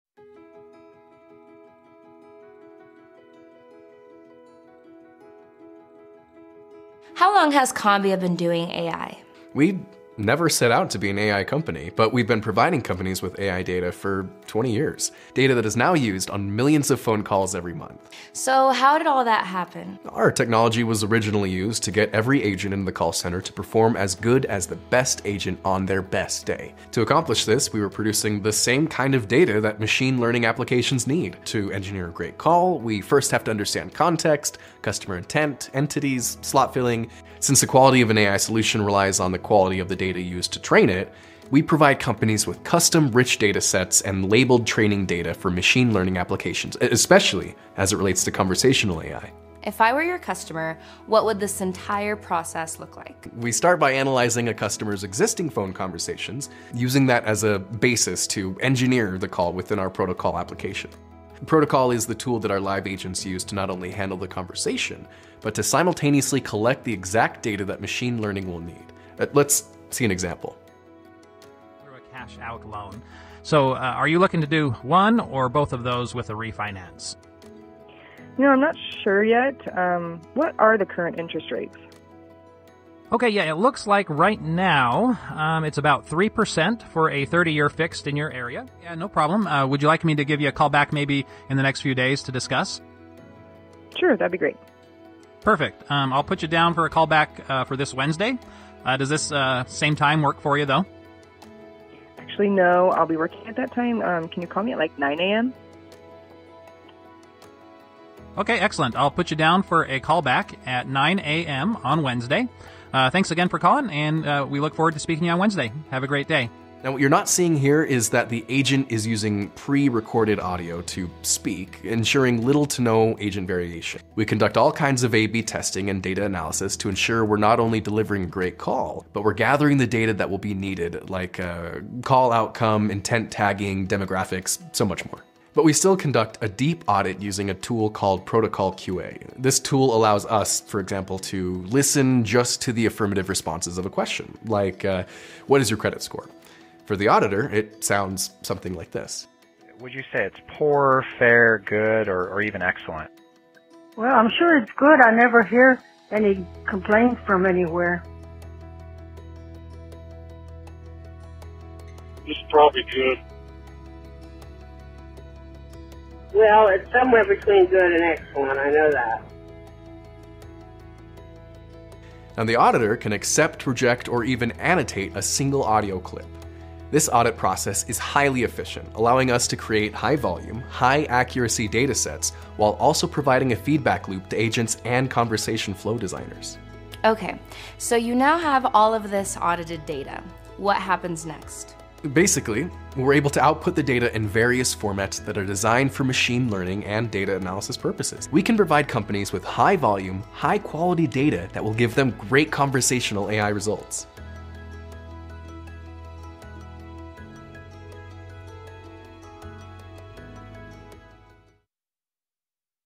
KomBea ProtoCall AI
We create rich, labeled datasets with audio samples and metadata such as call context, outcome, customer demeanor, accent, age and gender.

The quality of your AI solution relies heavily on the quality of the data used to train it. Our team harnesses intelligence, skills, technology, and cultural knowledge to create custom rich datasets and labeled training data for your machine-learning applications, Conversational AI, AI Bots, and Intelligent Platforms.

KomBea has designed and improved organization’s customer phone interactions for more than 20 years. Our technology and expertise are used on millions of phone interactions each year. We provide organizations with rich, labeled training data for their Conversational AI initiatives using their own customer phone interactions. Our high-volume, high-accuracy datasets include Intents, Entities, Utterances and Context. We provide you with:

Full audio with text transcription
Call context and outcome
Utterances with intent tags and slot filling
Metadata tags such as: Demographics (age, gender, accents), Sentiment (sarcasm, anger, frustration), Environment (background noise, latency, volume)